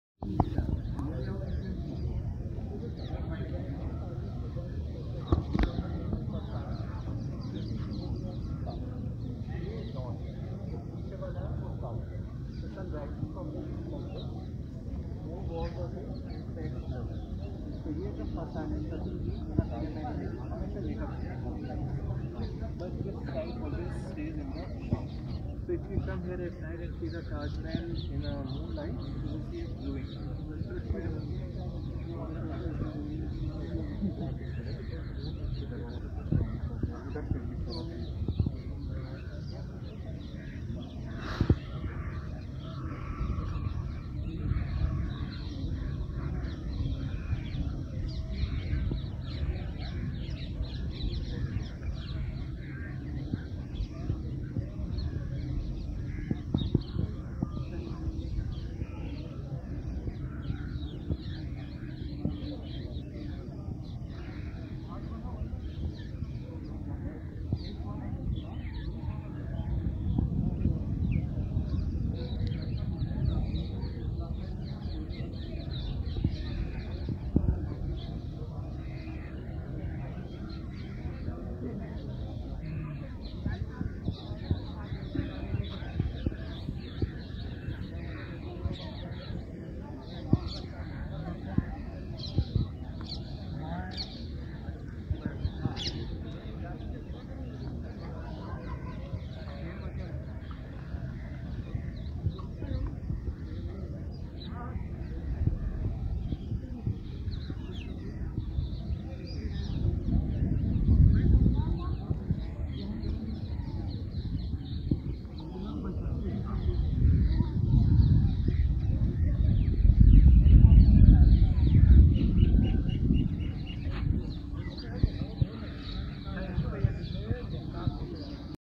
Does anyone live at the Taj Mahal?/Happy vlogs
Does anyone live at the Taj Mahal?
Who's Currently Buried in the Taj Mahal? This gigantic mausoleum only houses two people's remains: Mumtaz Mahal's and Shah Jahan's.